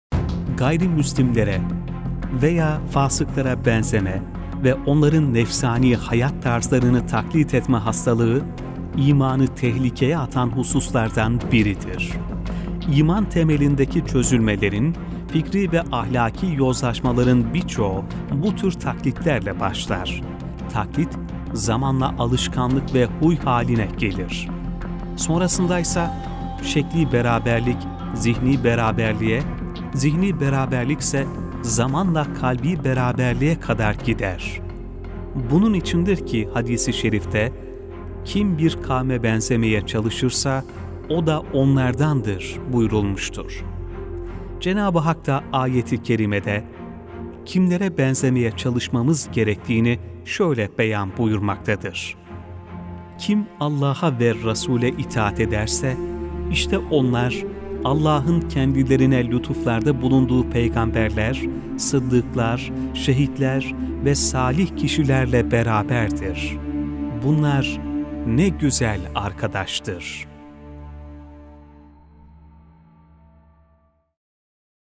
Kim Bir Kavme Benzerse Onlardandır
İmanınızı tehlikeye atmayın! Bakın Peygamber Efendimiz nasıl uyarıyor: “Kim bir kavme benzerse onlardandır.” buyuruyor. (Ebû Dâvud, Libâs, 4/4031)"